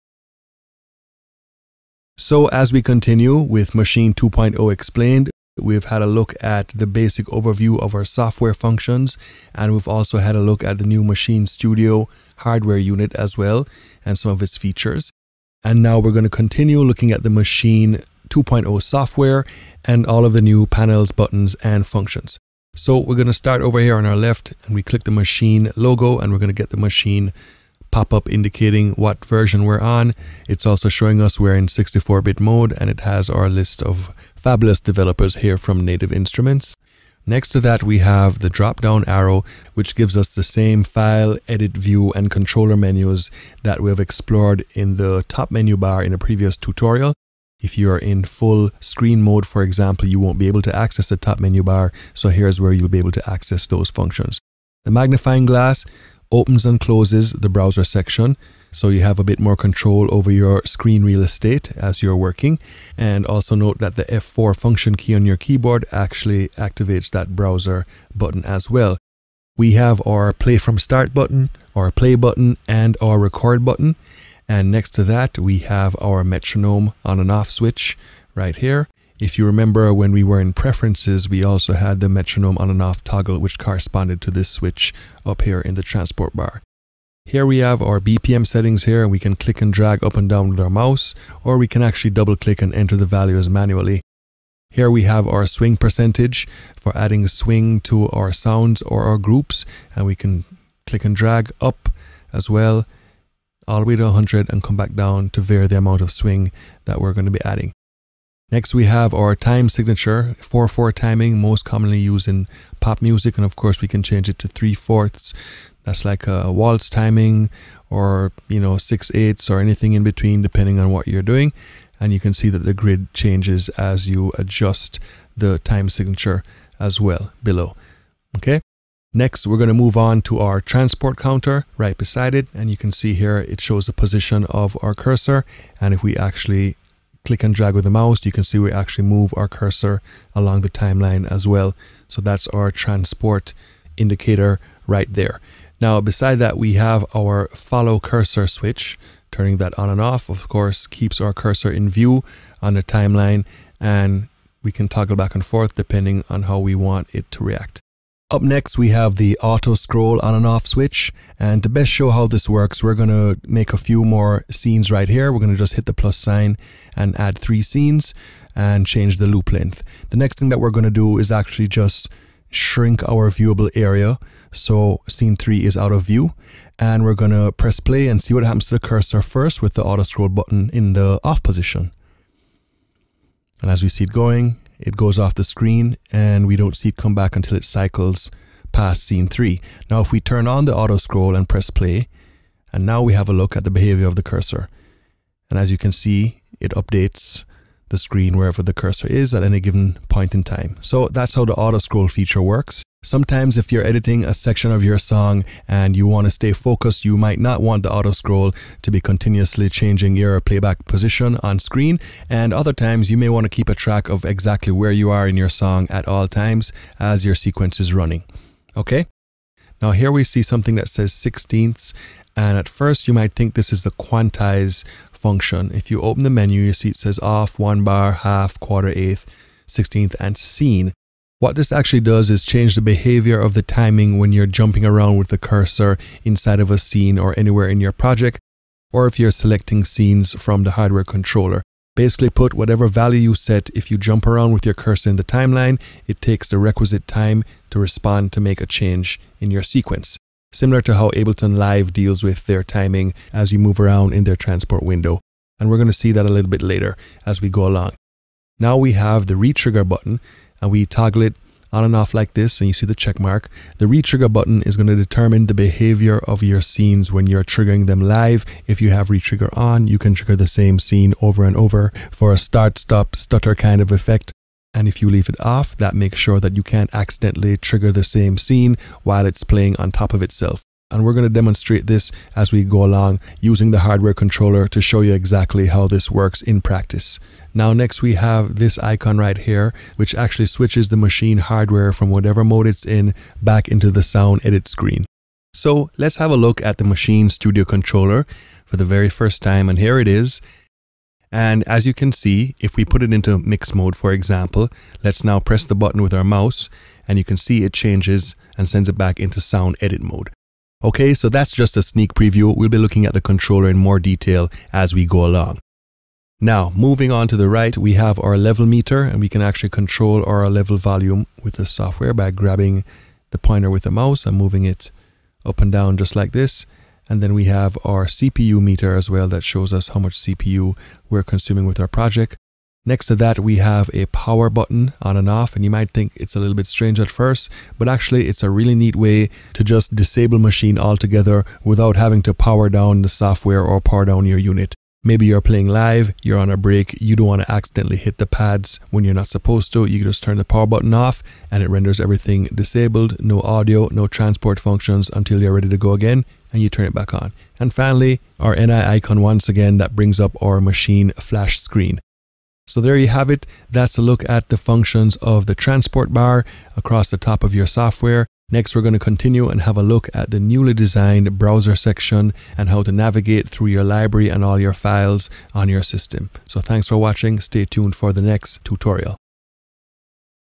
Transport Bar Explained (MASCHINE 2.0 Explained)
This area contains some very basic but also a few unique control settings that are easily misunderstood. Worthy of explanation.